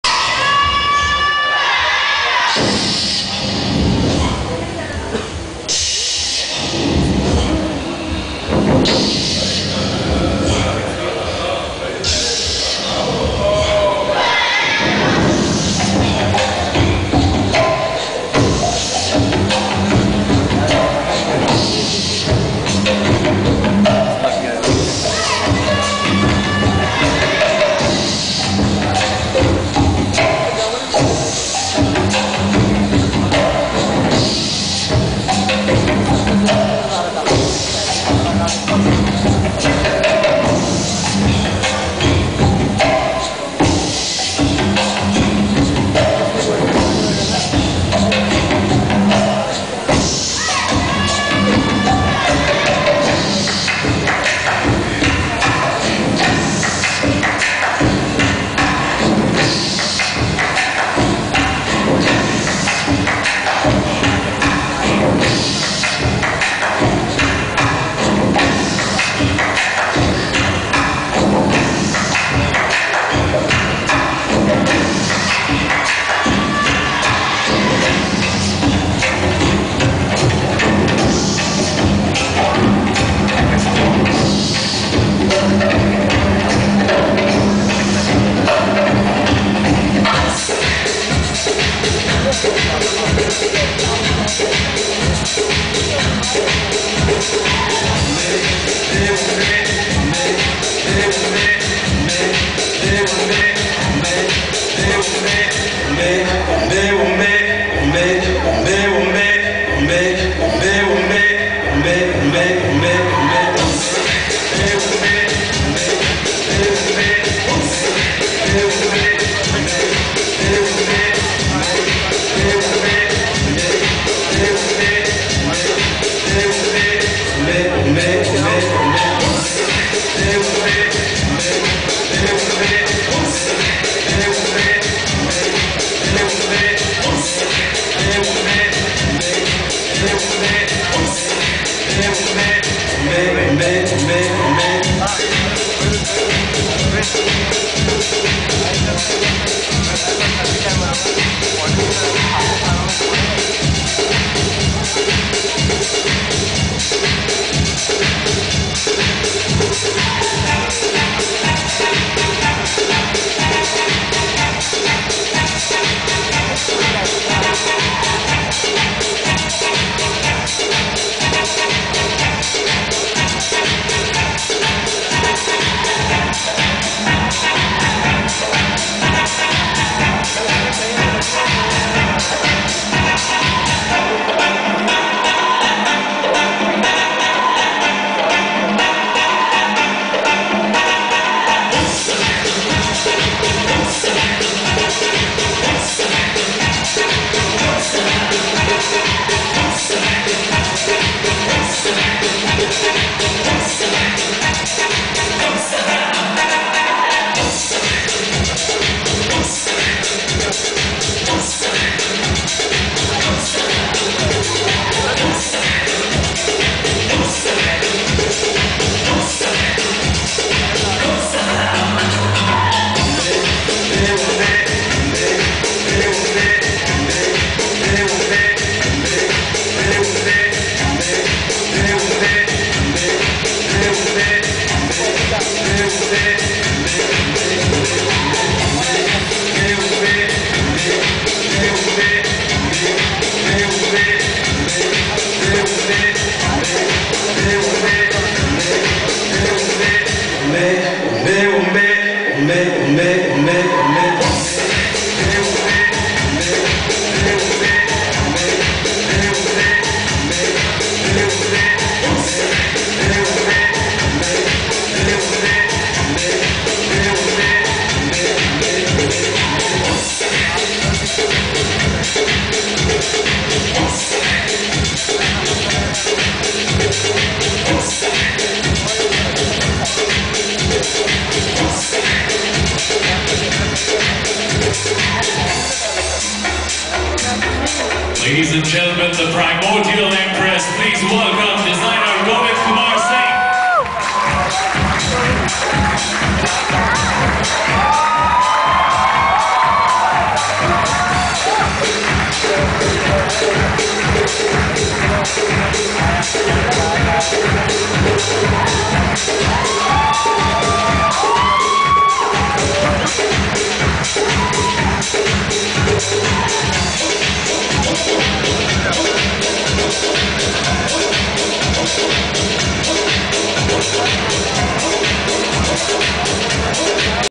Primordial Empress by Govind Kumar Singh.MPG
National Institute of  Fashion Technology (NIFT) Bangalore , Design Showcase  2010 , Most Creative and Innovative Design Collection Award Wining Collection .